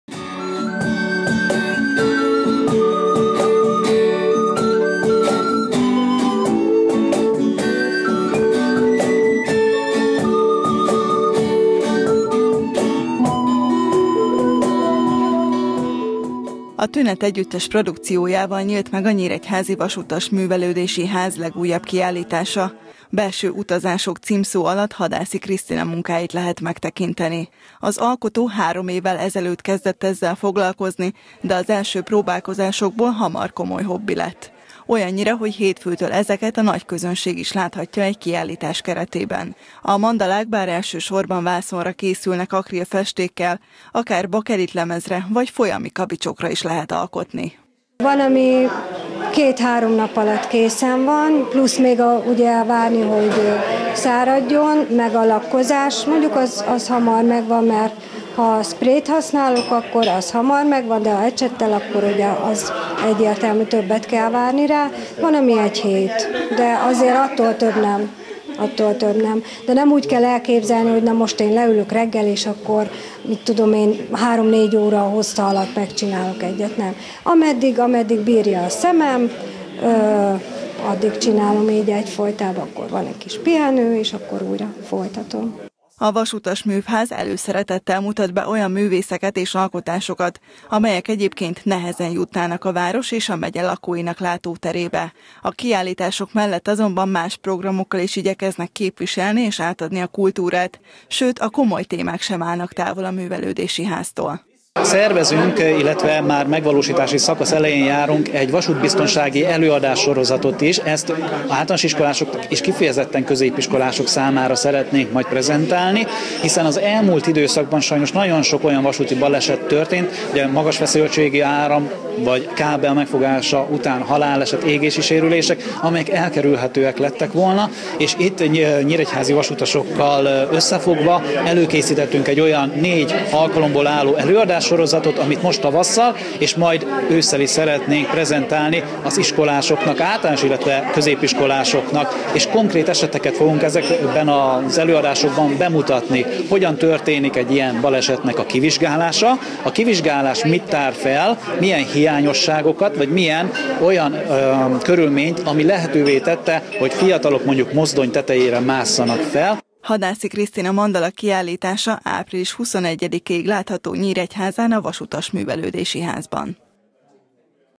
Különleges tárlat
A koronavírus-járvány miatti lezárások irányították a figyelmét Hadászi Krisztinának a mandala festésre. A különleges technikát három éve tanulta meg és most megnyithatta élete első kiállítását. A tárlat egy hónapig látható a Vasutas Művelődési Házban.